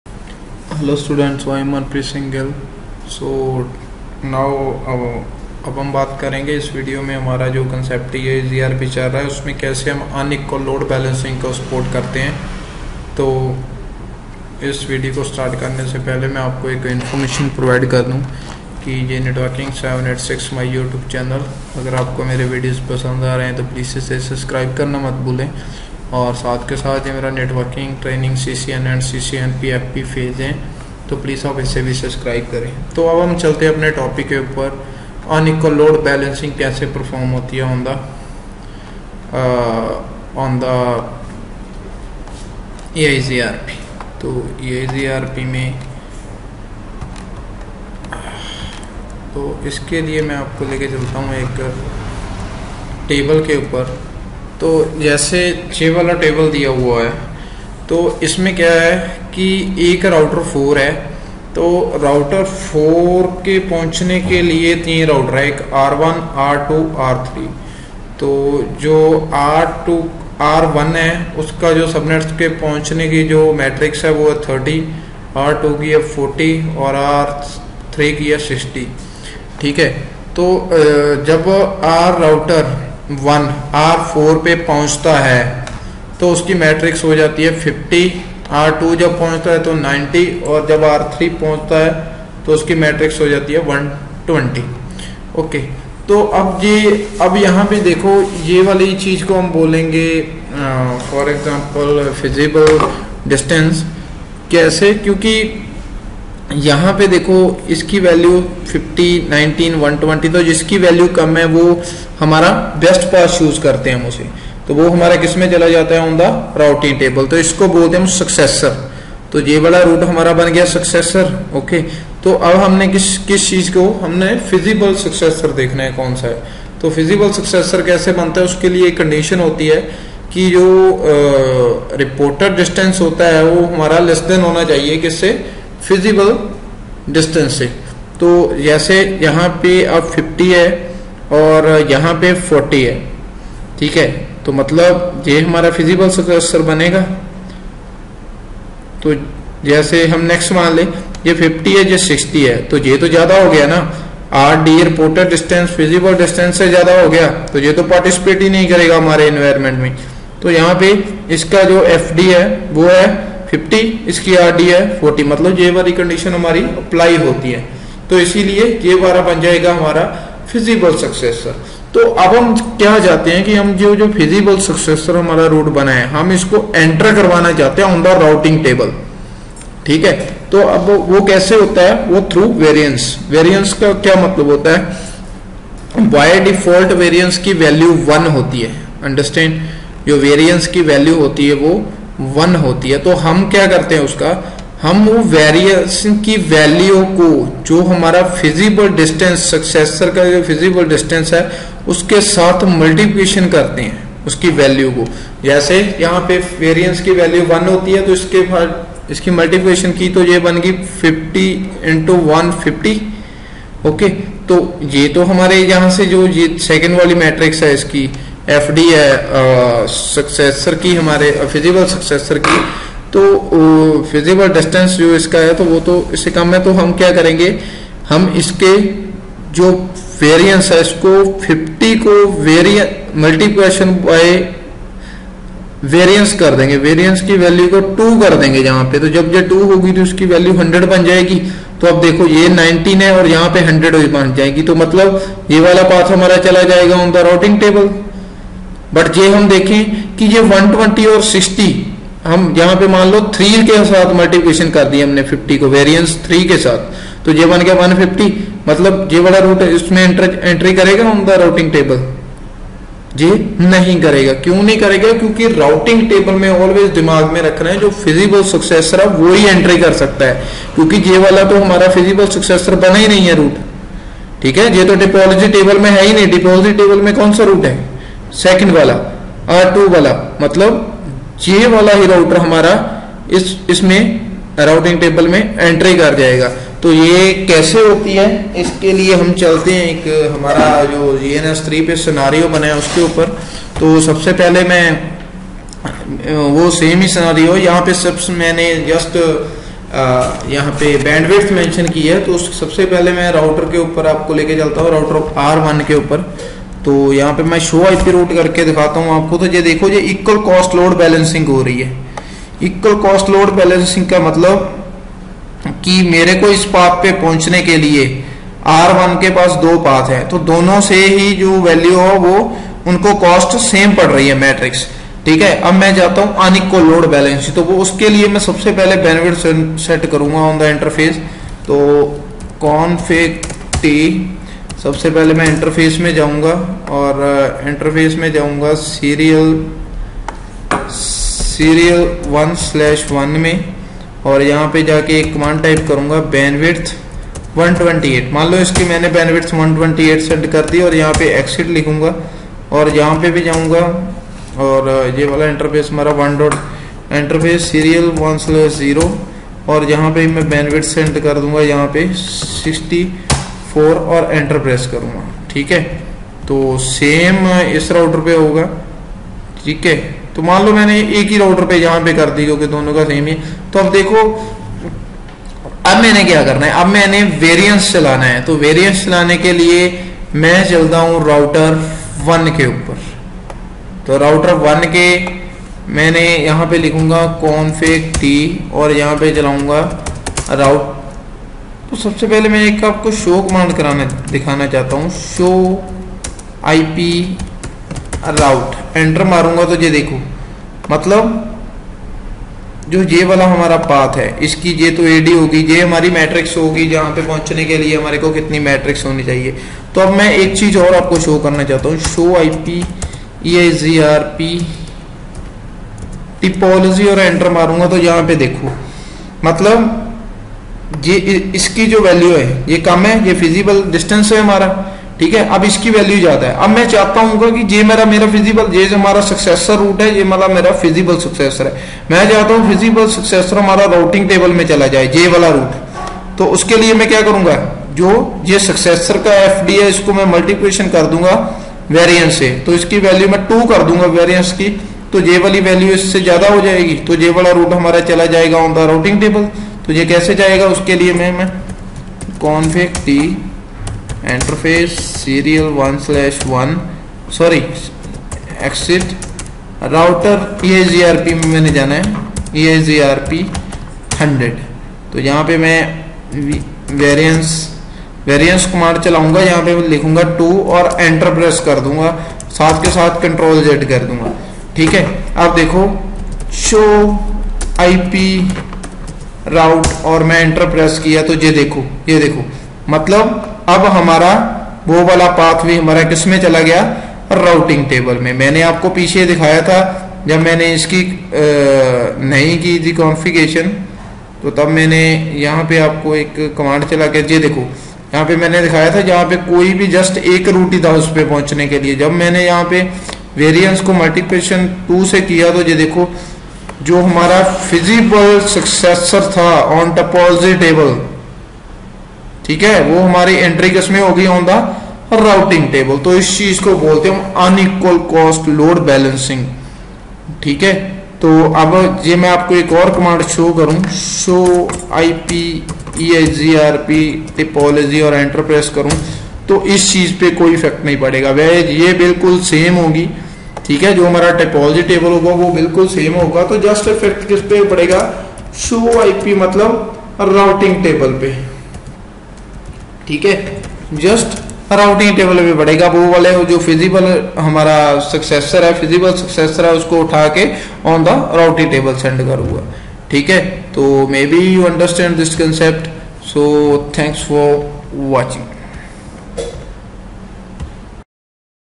EIGRP Un-Equal Cost Load Balancing Configuration Video-6 in Hindi.
CCNA EIGRP tutorial in Hindi By-Manpreet Gill shows Un-Equal Load Balancing in EIGRP Configuration on GNS3 in hindi on Cisco Router. CCNA 200-120 training Video in Hindi.